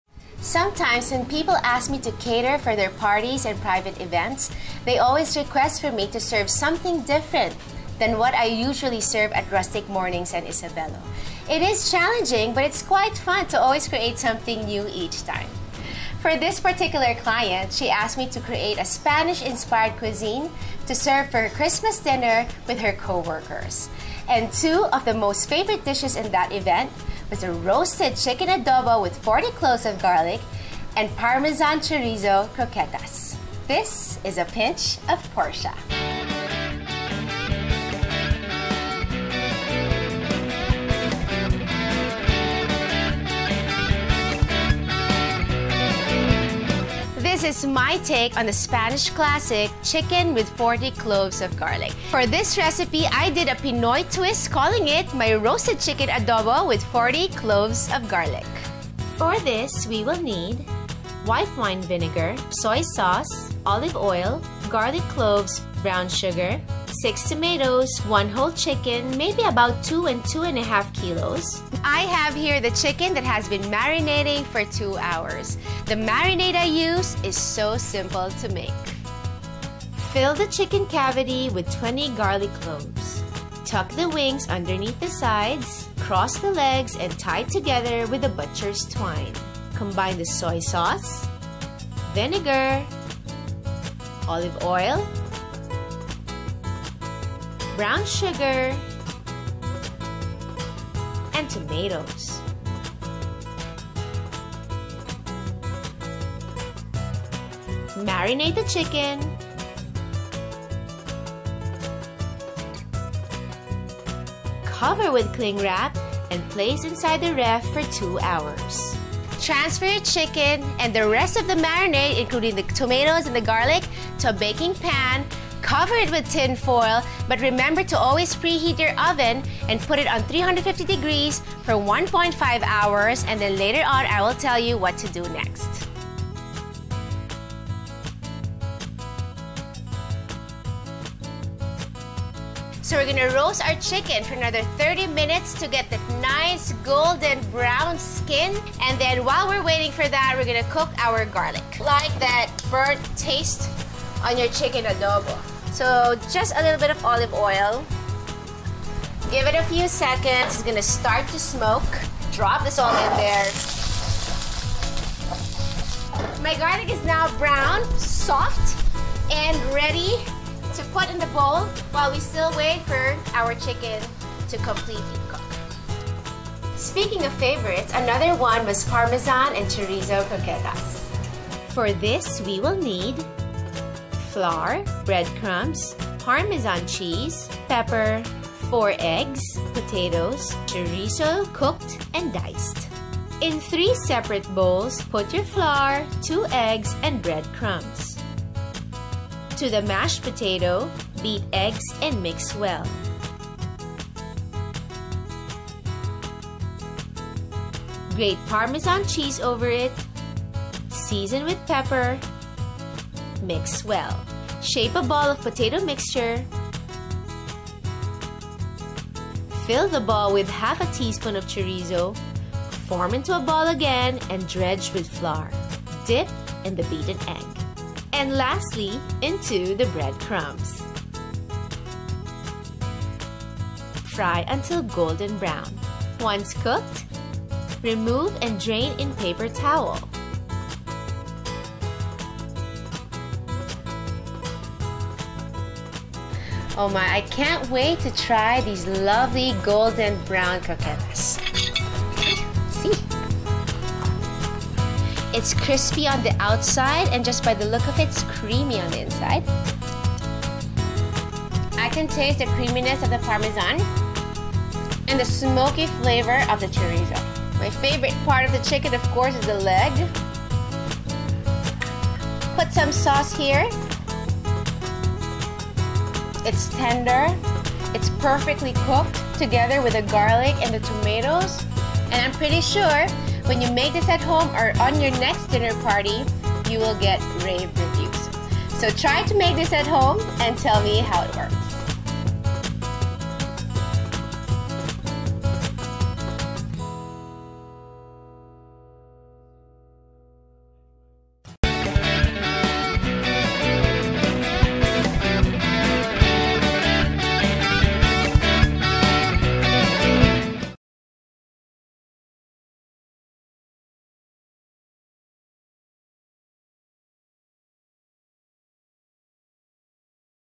Roasted chicken adobo with 40 cloves of garlic & Parmesan chorizo croquettas | APOP
Follow Portia as she shares her cooking secrets that you can easily learn in the comfort of your own home.